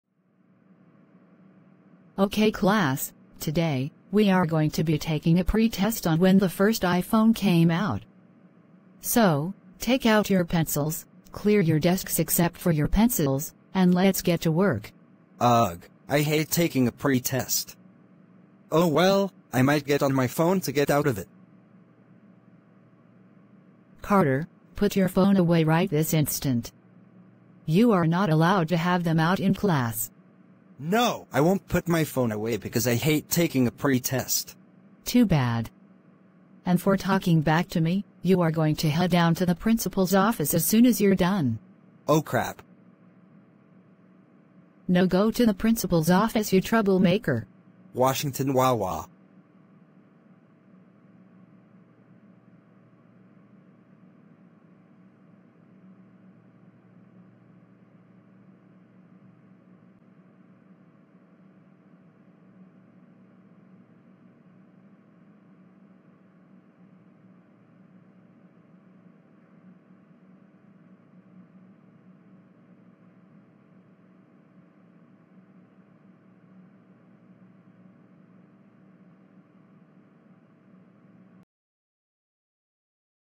Using your phone in class while your teacher is teaching in a nutshell
This is ment to teach people a lesson about what could happen if phones are used by students during your teachers teaching section.